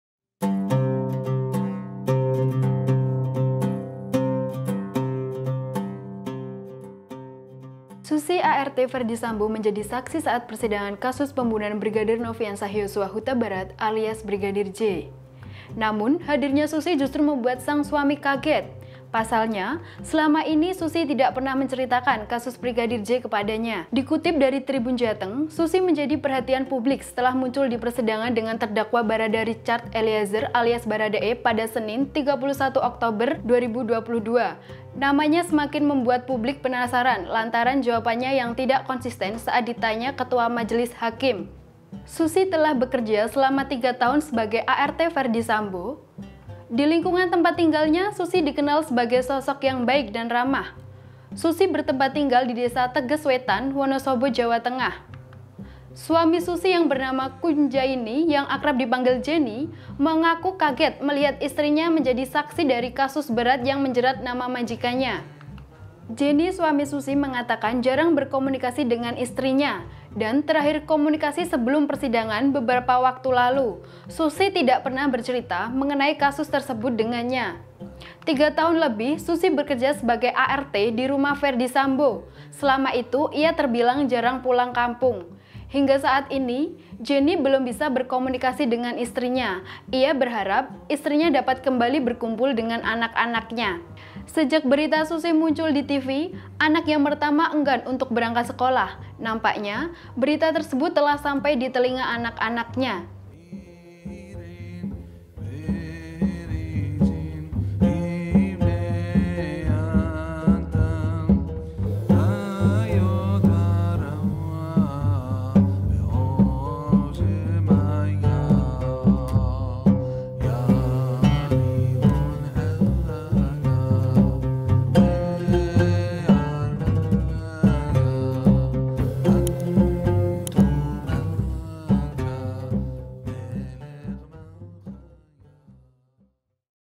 Istrinya Jadi Saksi di Persidangan Pembunuhan Brigadir J, Suami Susi ART Ferdy Sambo Kaget
Suaminya ART Ferdy Sambo Kaget Istrinya Jadi Saksi Persidangan Pembunuhan Brigadir J

Susi ART Ferdy Sambo menjadi saksi saat persidangan kasus pembunuhan Brigadir Nofriansyah Yosua Hutabarat alias Brigdir J.

Namun, hadirnya Susi justru membuat sang Suami kaget. Selama ini Susi tidak pernah menceritakan kasus Brigadir J kepadanya.